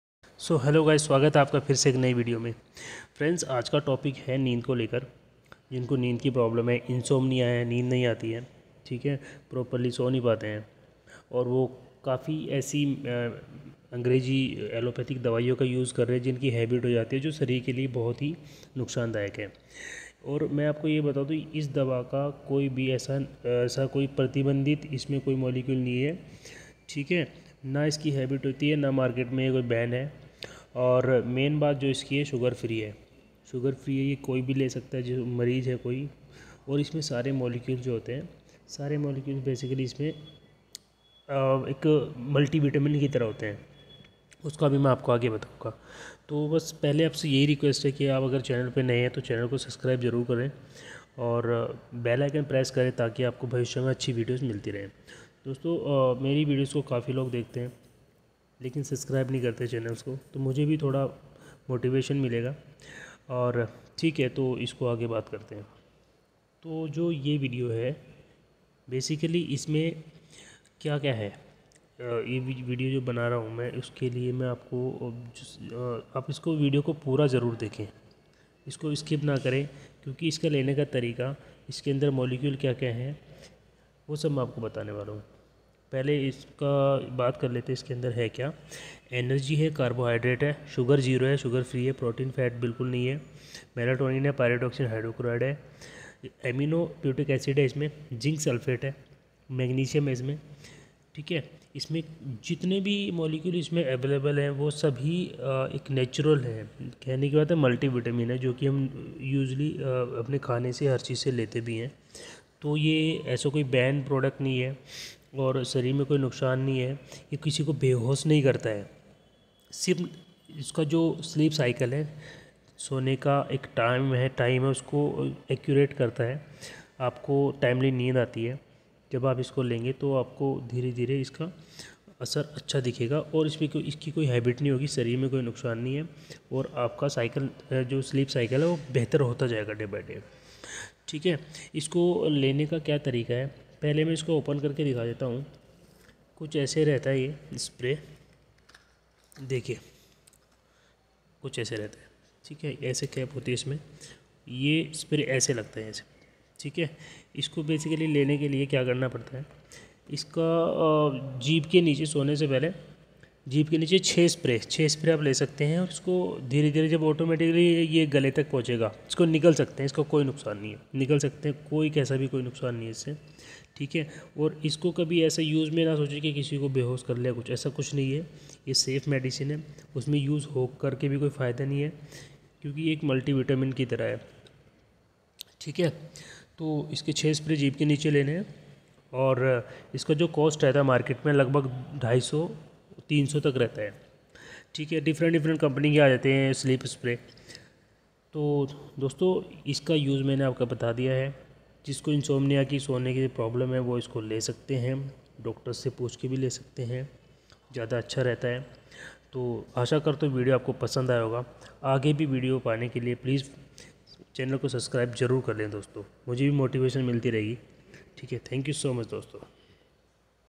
रात में नींद न आने का इलाज | insomnia treatment in hindi | neend na aane ki wajah ka ilaj. spray
इस दवा को लेने से अच्छी नींद आ जाती है
सोन का routine ठीक ho jata hai

Sugar free  hai।

no harmful chemicals

इसकी आदत भी नही होगी।

multivitamins se भरपूर hai